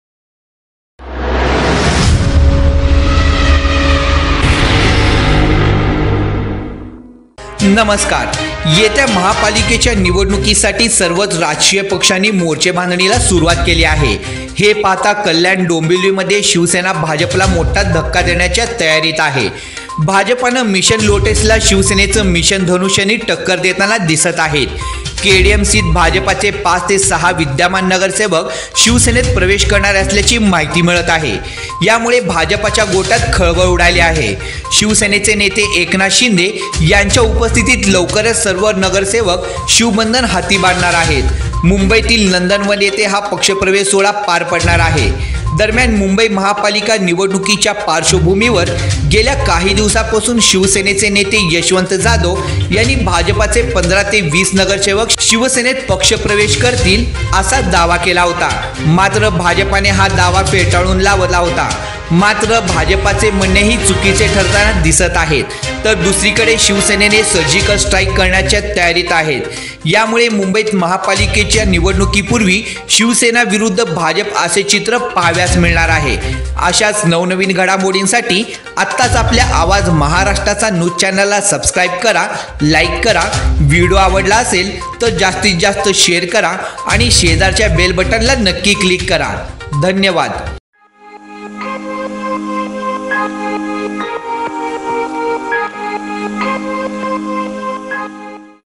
उध्दव ठाकरेंच मिशन "धनुष्यबाण" यशस्वी?भाजपच्या मिशन "लोटस" ला धक्का! Uddhav Thackeray Shivsena News
उध्दव ठाकरेंच मिशन "धनुष्यबाण" यशस्वी?भाजपच्या मिशन "लोटस" ला धक्का! Big News Today#AwaazMaharashtracha#Tv9Marathi#maharashtraupdates#uddhavthackeray